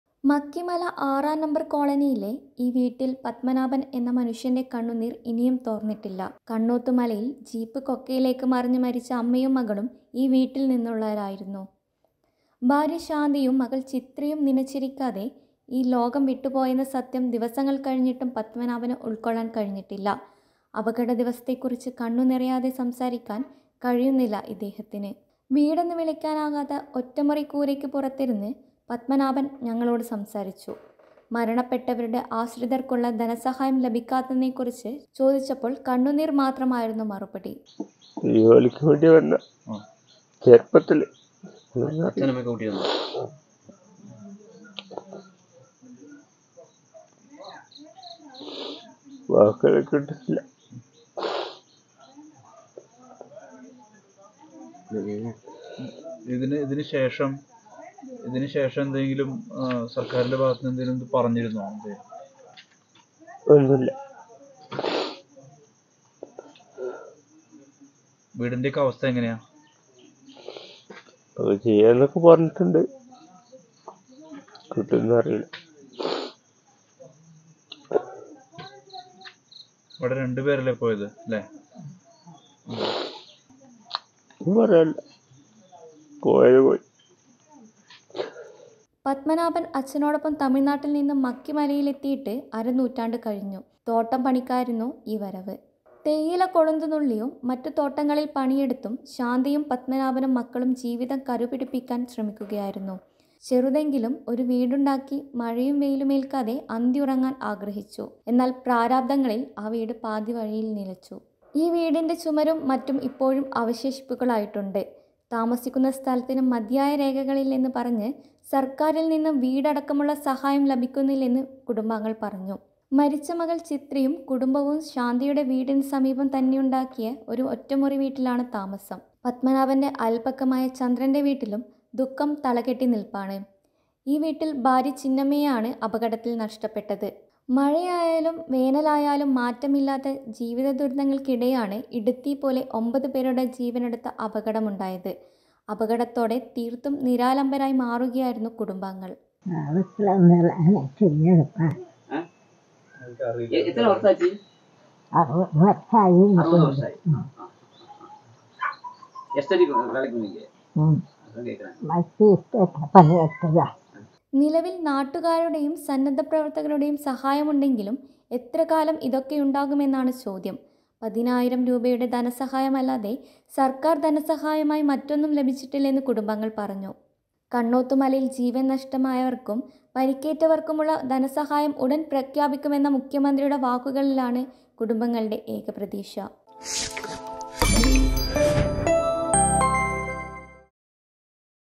Kannothumala Accident: ഒറ്റമുറി കൂരയിൽ ഇനി പത്മനാഭൻ തനിച്ച്
തീരാ കണ്ണീരോടെ മക്കിമല നിവാസികൾ